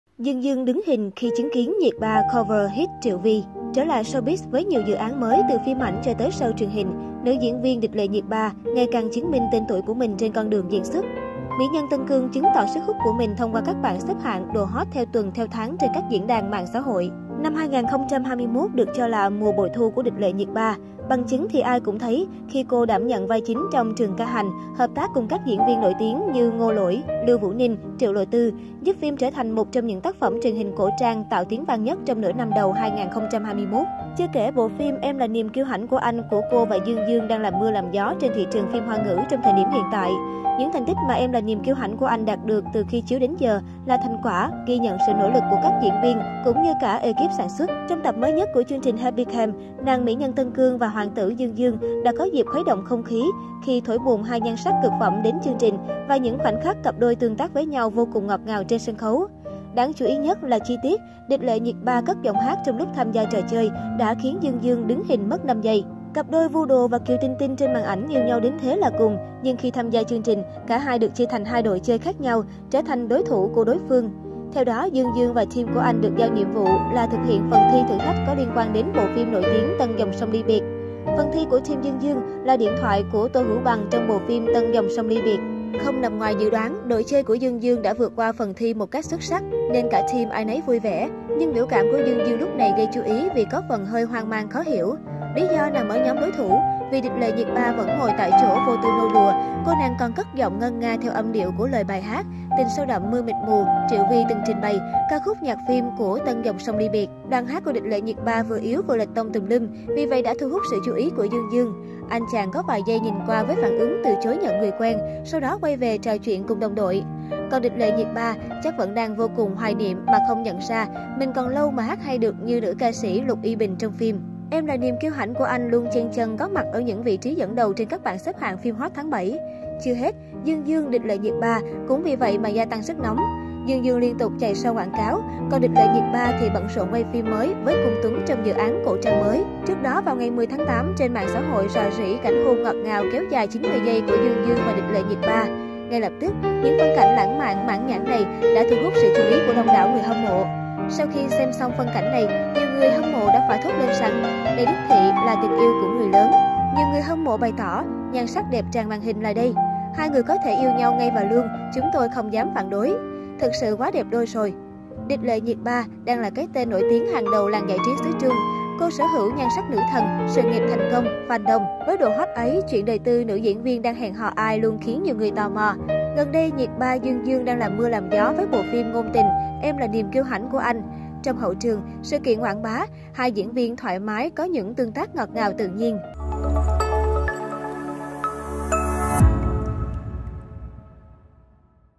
Dương Dương "đứng hình" khi chứng kiến Nhiệt Ba cover "hit" Triệu Vy
Trở lại showbiz với nhiều dự án mới từ phim ảnh cho tới show truyền hình, nữ diễn viên Địch Lệ Nhiệt Ba ngày càng chứng minh tên tuổi của mình trên con đường diễn xuất. Mỹ nhân Tân Cương chứng tỏ sức hút của mình thông qua các bảng xếp hạng, độ hot theo tuần, theo tháng trên các diễn đàn MXH.

Năm 2021 được cho là mùa "bội thu" của Địch Lệ Nhiệt Ba. Bằng chứng thì ai cũng thấy khi cô đảm nhận vai chính trong Trường Ca Hành hợp tác cùng các diễn viên nổi tiếng như Ngô lỗi, Lưu Vũ Ninh, Triệu Lộ Tư giúp phim trở thành một trong những tác phẩm truyền hình cổ trang tạo tiếng vang nhất trong nửa năm đầu 2021.